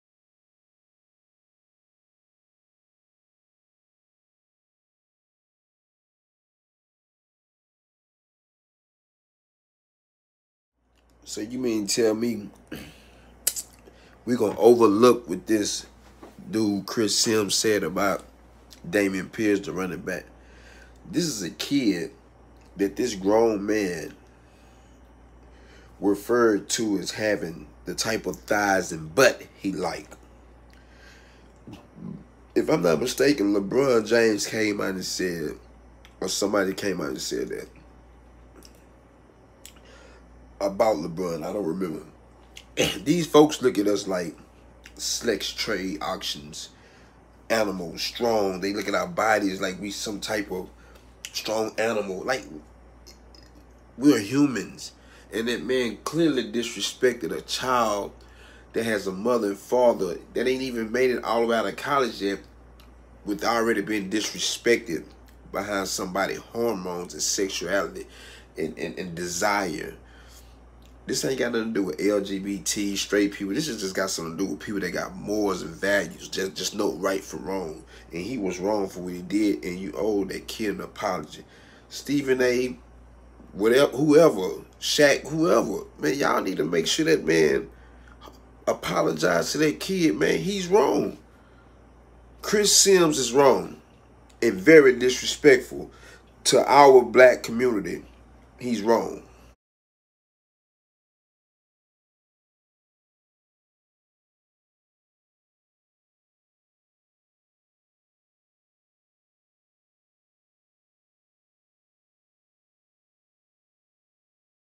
Rb Dameion Pierce was violated sexual an verbally abused an disrespected by Chris Simms X QB (watch
If we don't confront this then who will ..This guy chris needs to apologize to this kid he didn't deserve that ..please share this so it can get awareness thanks to my supporters